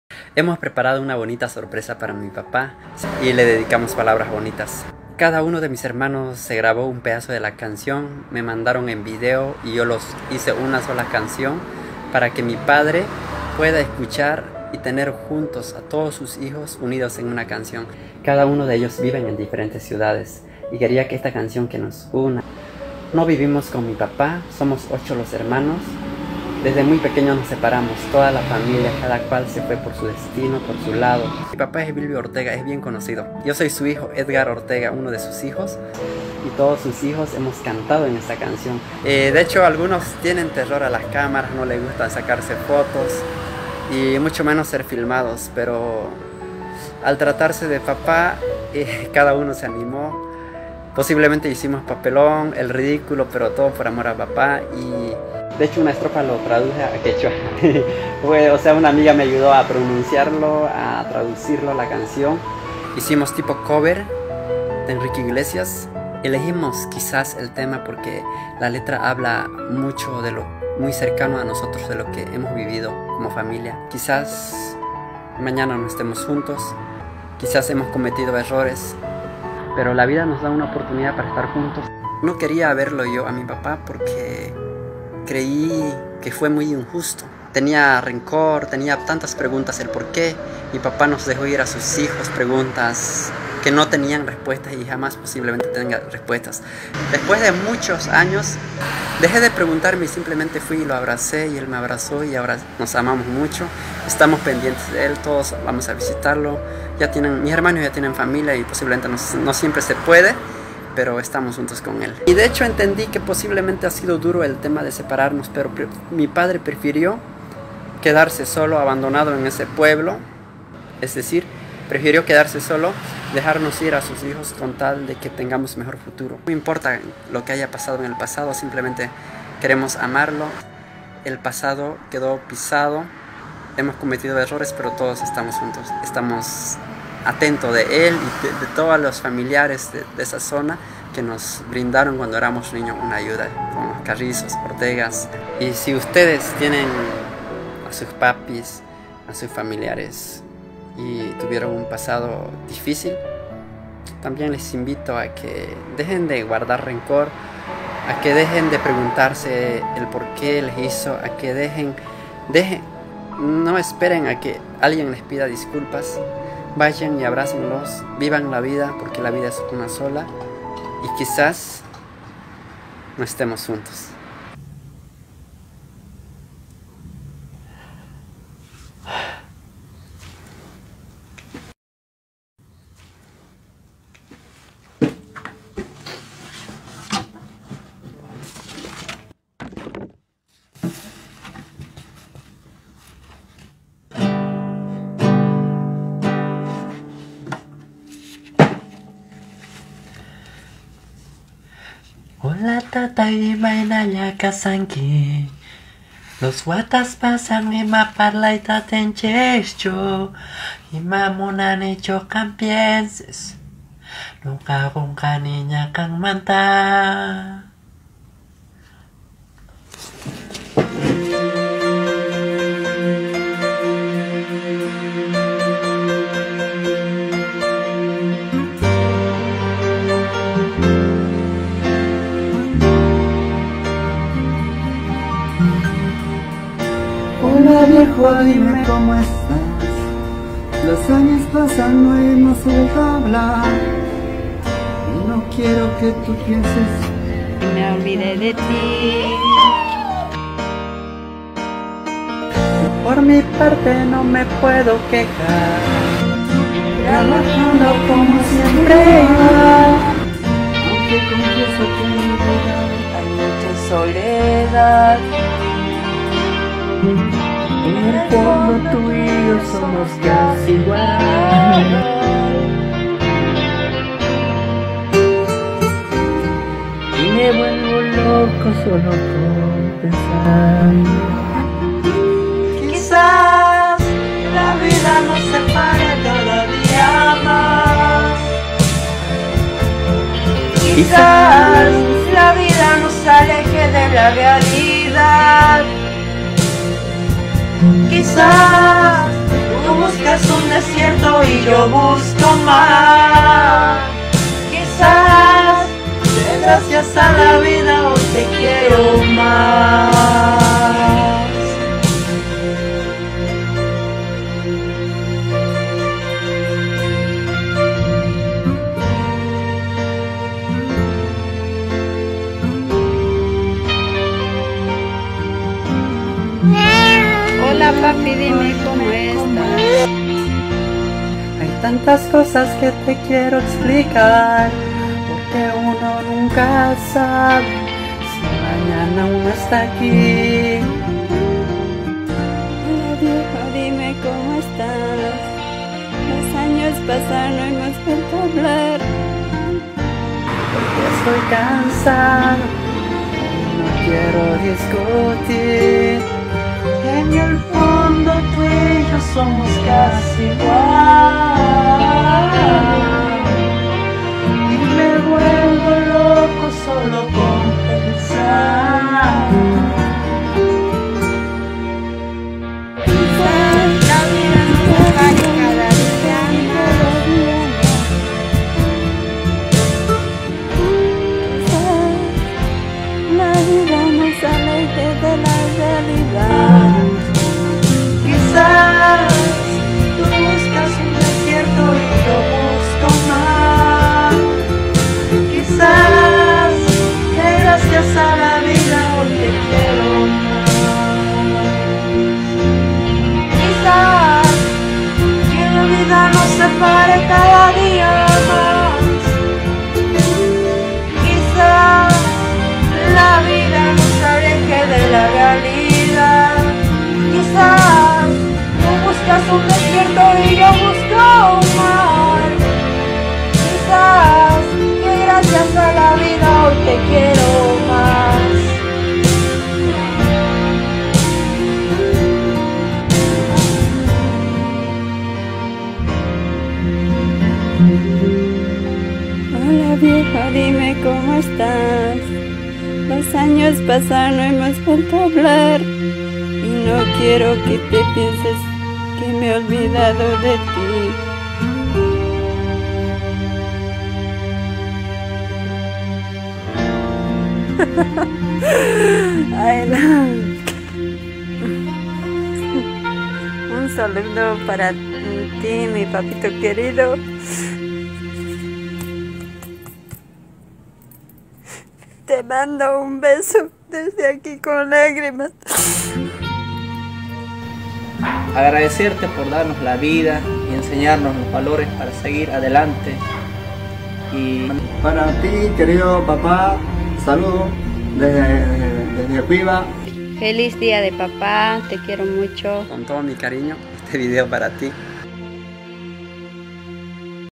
EL VIDEO DE PAPA QUE HIZO LLORAR A MUCHOS
una bonita sorpresa para papá conmovió al MUNDO entero.